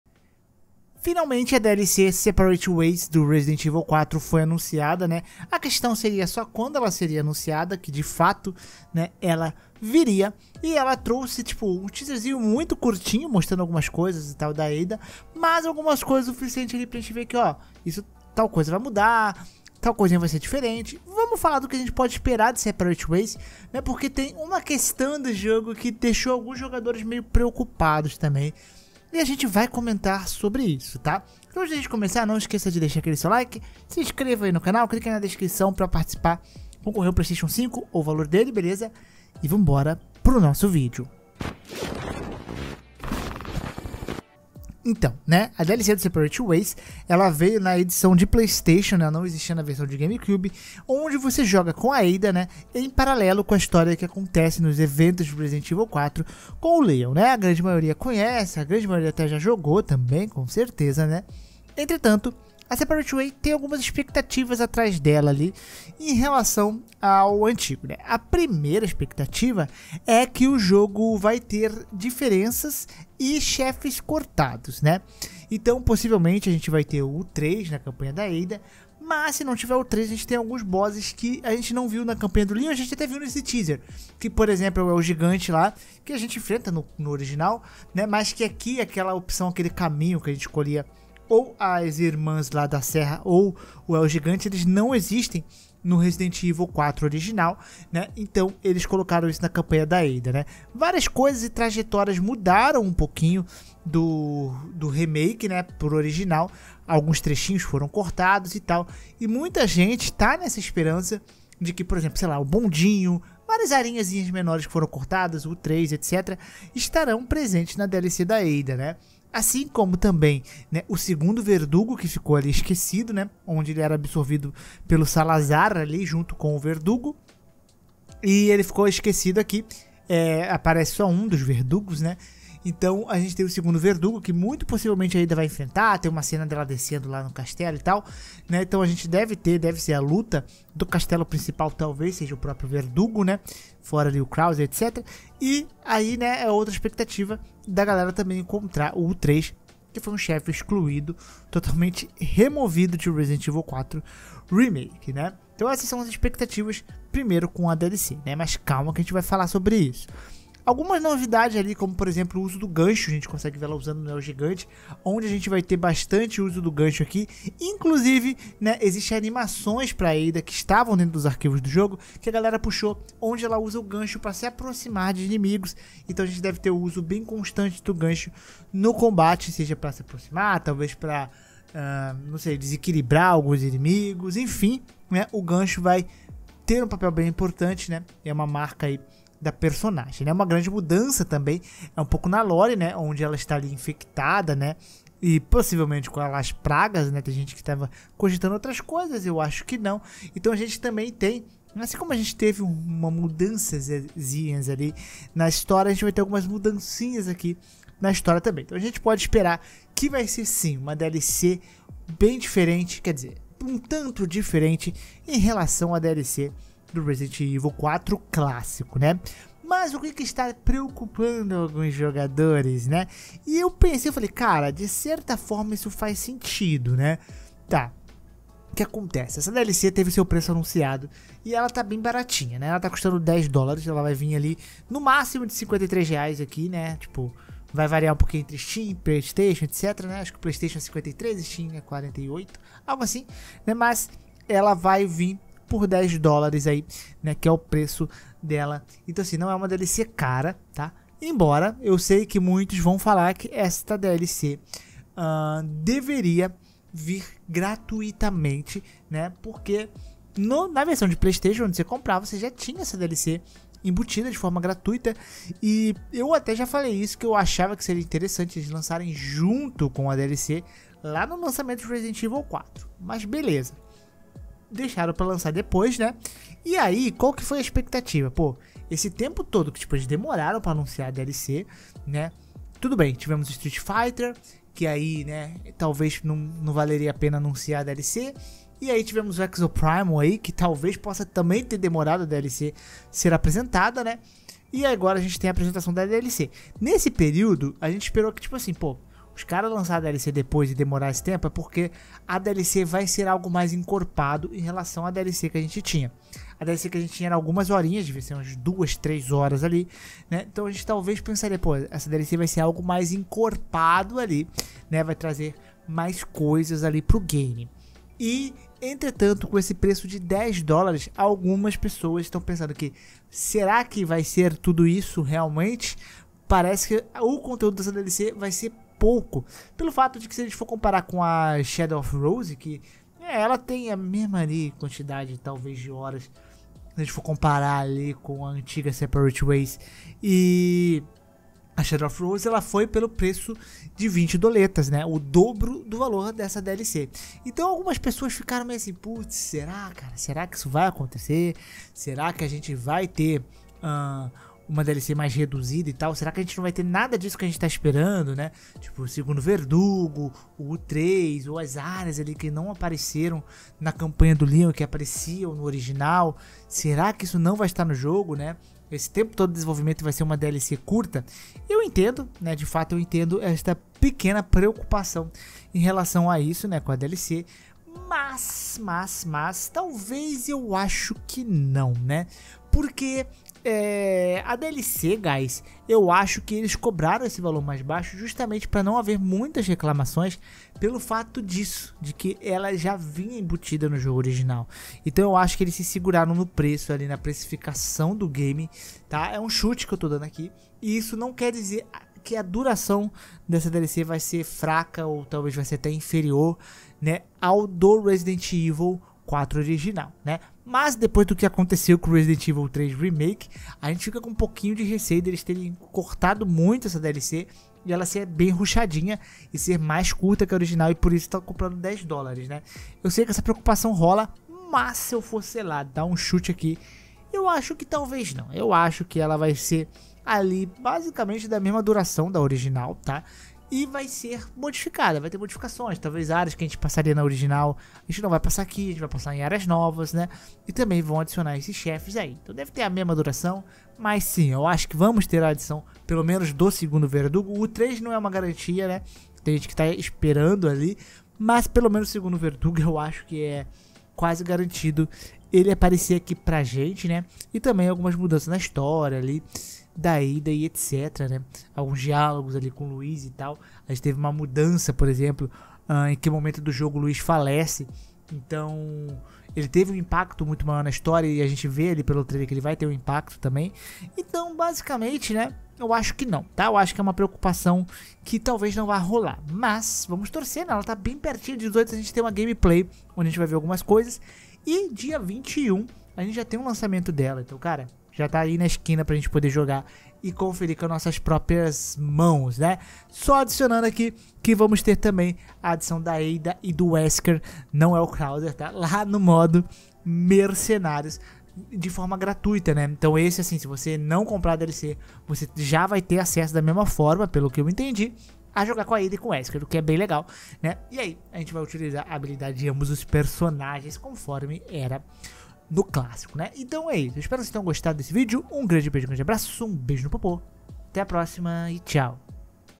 DLC de Resident Evil 4 - Detalhes e MUDANÇAS, e ISSO É PREOCUPANTE? (Separate Ways)
Sobre a dlc do Re4 Remake

➤ PSN: hadessama94

00:00 O que vai mudar
03:40 Preocupações